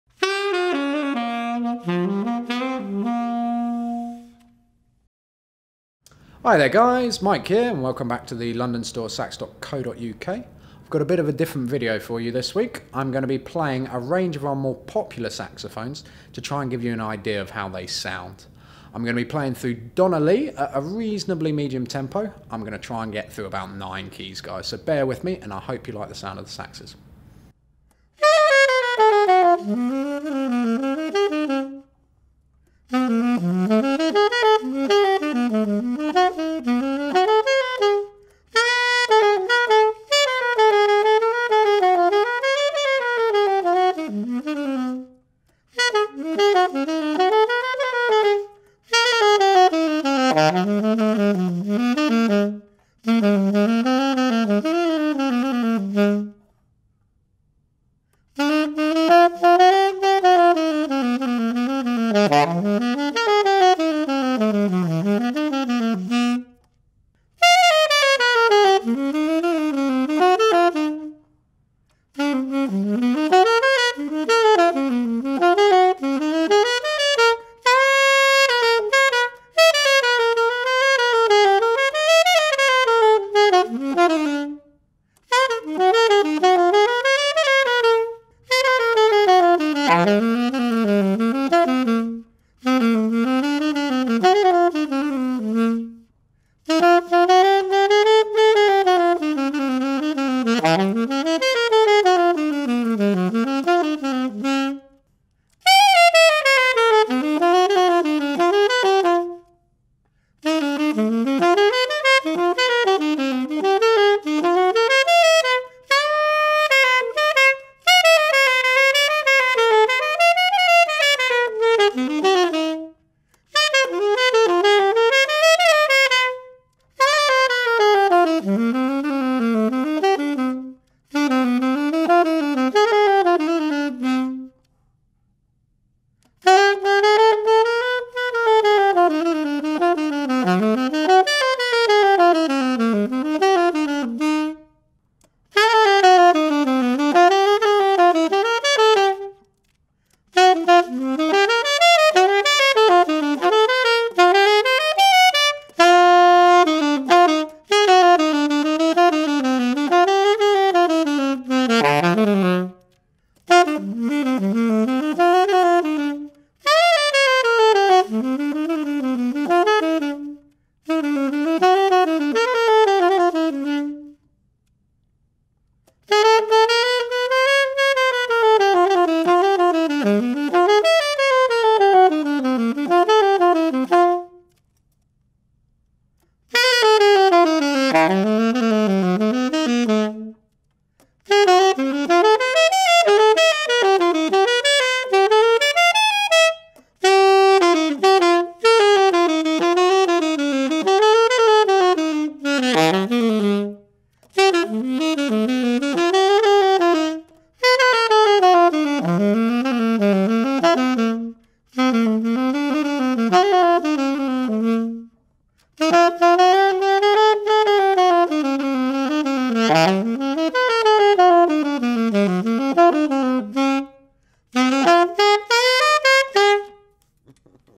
Alto Saxophone Showdown
Mike Brown plays through a handful of our most popular alto saxophones.

List of saxophones played (in order of appearance):
Selmer Paris Reference 54
Selmer Paris Mark VI
Yanagisawa AWO10
Cannonball Vintage Reborn
Conn-Selmer Premiere PAS-380V
Eastman 52nd Street
Keilwerth MKX
Signature Custom RAW XS
Yamaha YAS-82Z
P.Mauriat PMXA-67RUL

Mike is using a D'Addario Select Jazz Alto mouthpiece.